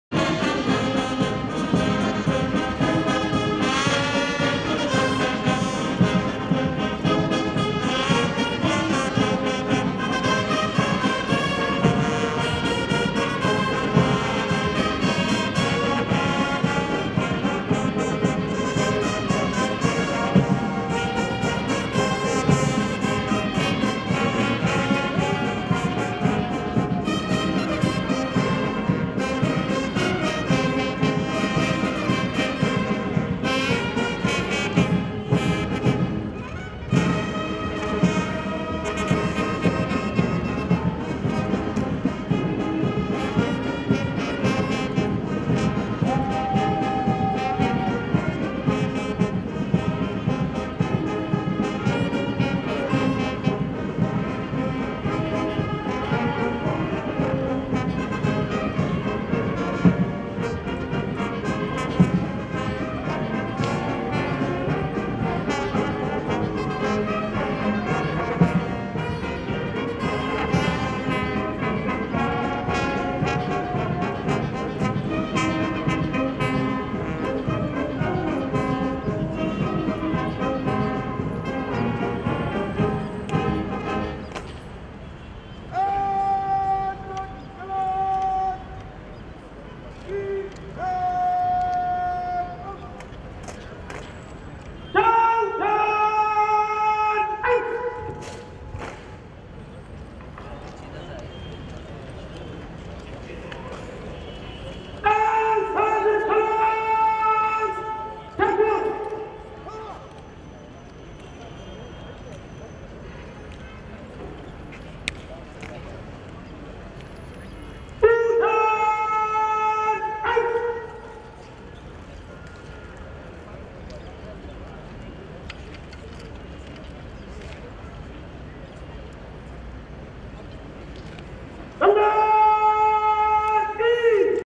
This Is How Colours Mathes On Military Parades
It is a clip during the Independence Day Celebration 2019 in Abuja, Nigeria.